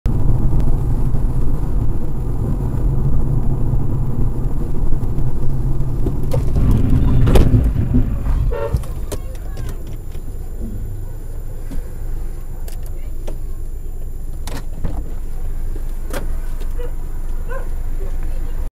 ДТП 2015 октябрь (пешеходы, Новосибирск) видеорегистратор
24 октября 2015 г. женщина выходя из автобуса, резко стала перебегать полосу встречного движения. С женщиной всё в порядке!